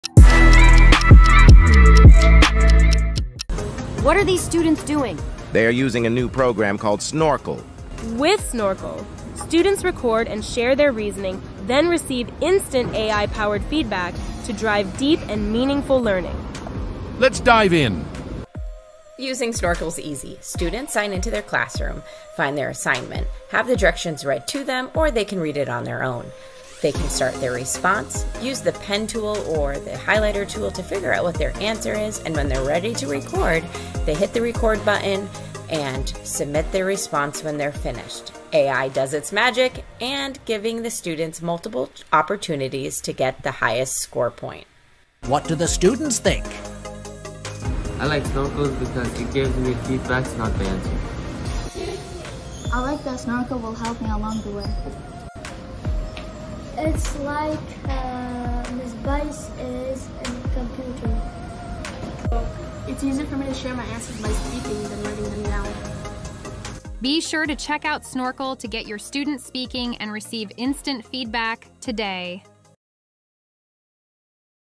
1 Minute Tech Tech Tip- Snorkl for AI-Powered Student Feedback
This tech tip shows how to use Snorkl with your students to give AI-powered feedback.